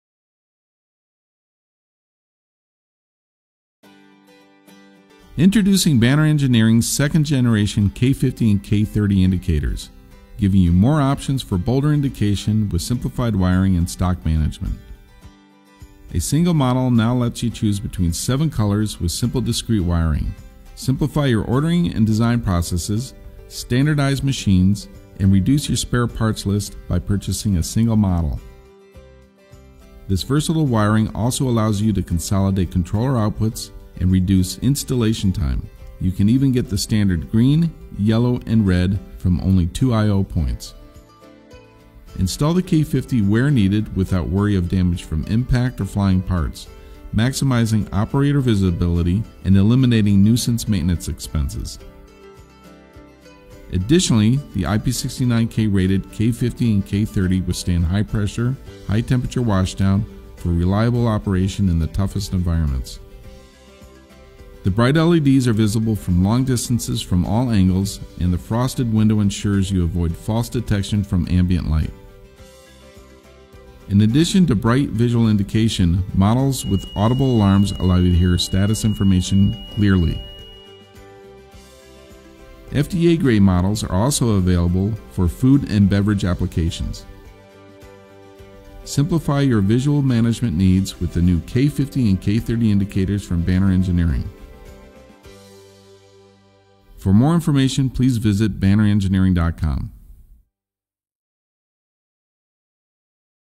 Banner 2nd Gen K30 & K50 Pro Series Programmable Domed Indicators— Allied Electronics & Automation
The second generation of K50 and K30 domed indicators from Banner Engineering give you more options for bolder indication with simplified wiring and stock management. Choose between seven colors with a single, rugged model.

Transcript

Introducing Banner Engineering's second-generation K50 and K30 indicators giving you more options for bolder indication with simplified wiring and stock management.

A single model now lets you choose between seven colors with simple discrete wiring.

Simplify your ordering and design processes, standardize machines, and reduce your spare parts list by purchasing a single model.

This versatile wiring also allows you to consolidate controller outputs and reduce installation time.

You can even get the standard green, yellow, and red from only two i/o points.

Install the K50 where needed without worry of damage from impact or flying parts, maximizing operator visibility and eliminating nuisance maintenance expenses.

Additionally, the IP69K rated K50 and K30 withstand high-pressure, high-temperature washdown for reliable operation in the toughest environments.

The bright LEDs are visible from long distances from all angles and the frosted window insures you avoid false detection from ambient light.

In addition to bright visual indication, models with audible alarms allow you to hear status information clearly.

FDA grade models are also available for food and beverage applications.

Simplify your visual management needs with the new K50 and K30 indicators from Banner Engineering.